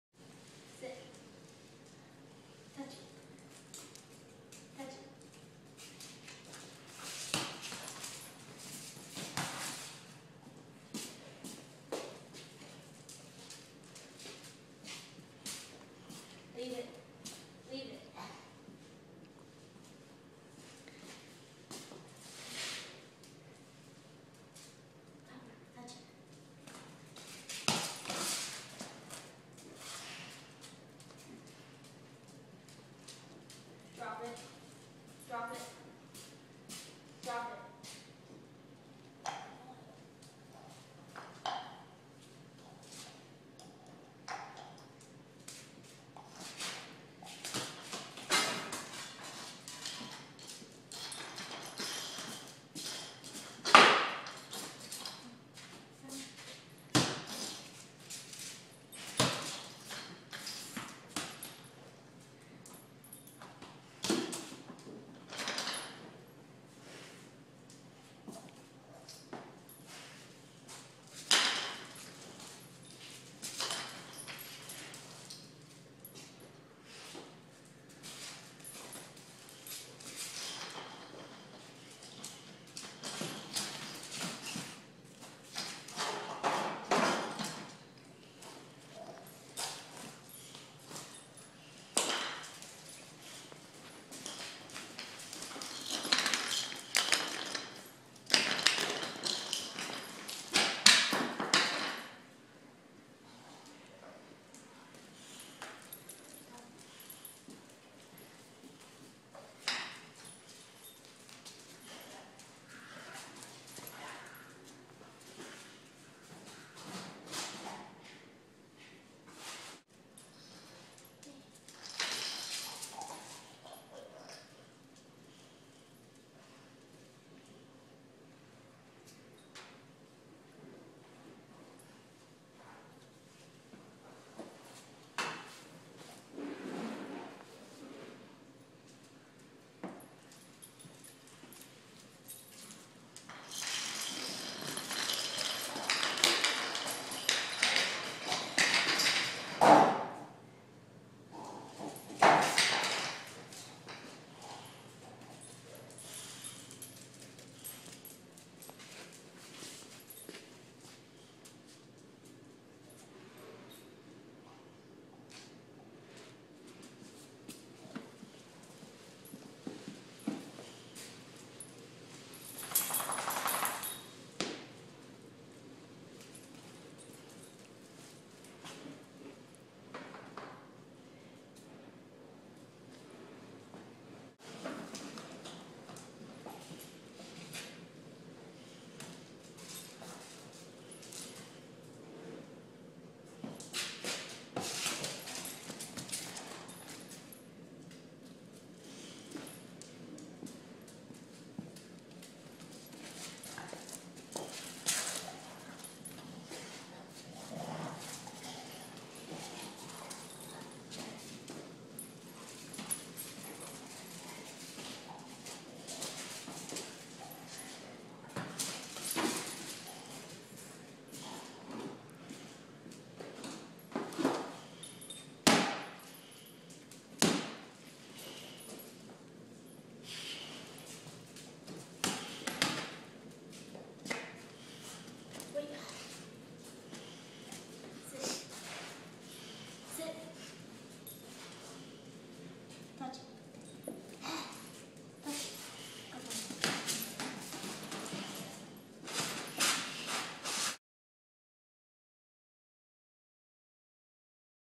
PitBull and Boxer Playing
Spencer the Blue Nose Pitbull and Bruno the goofy brown brindle Boxer play with their balls and a bone. Watch how goofy Bruno the Boxer gets. He starts playing with his bone all by himself. Notice how Spencer the Pitbull wants the bone, but he won't go and take it from Bruno. However the first chance he gets he gets the bone on his own! He's no dummy! Forget that ball, he wants the bone! Sara taught Spencer to "Touch it" with his snoot. Spencer gets the ball when he does this on command, but he does not get it when he uses his teeth to touch the ball. Only the snoot. Spencer is good at this trick. If you hold your hand out to him and say "Touch it" he'll give you a "Snoot High Five!". See more of Bruno and Spencer at BrunoTheBoxer.com